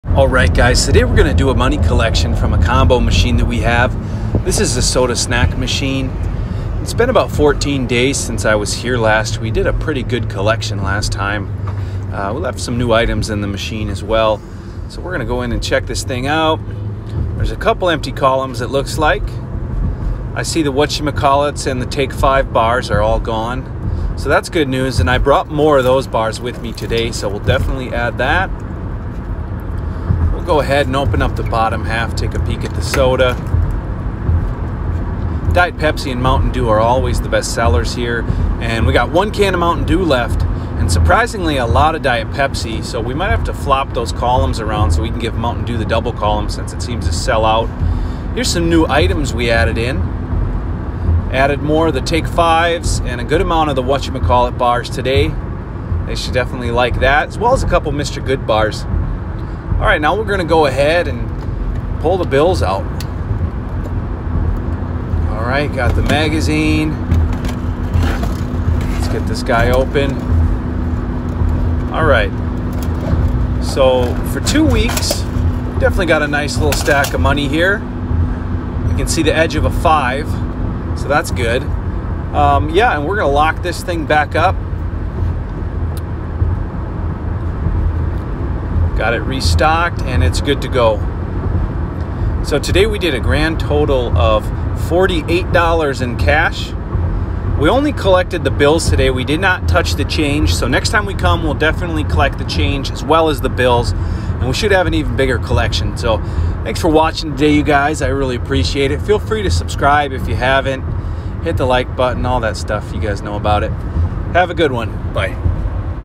Money Collection 💴 Vending Machine - 14 days BILLS ONLY
Money Collection from one of our vending machines. Another day in the vending machine business!
- Top line thrift find